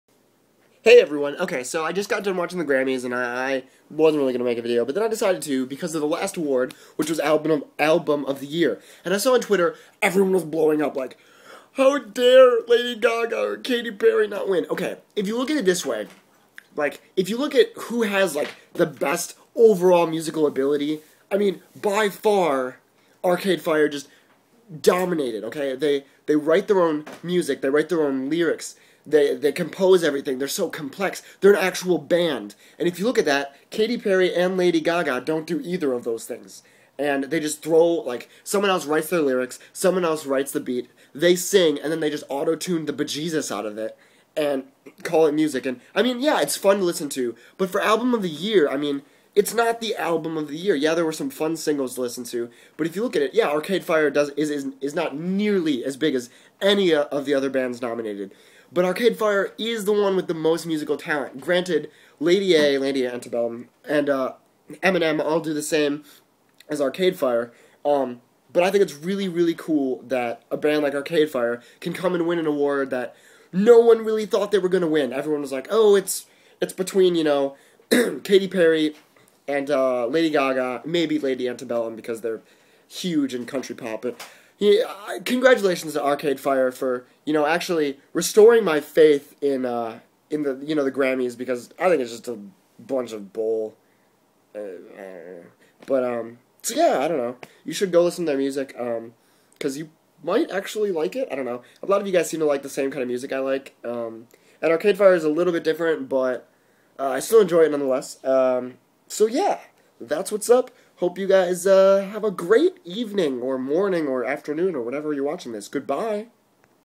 Mainstream Media v. Arcade Fire
Congrats Arcade Fire. Tonight, they proved that you don't have to have the biggest fan base to win Album of the Year. A band with Talent deserves that award. Great work.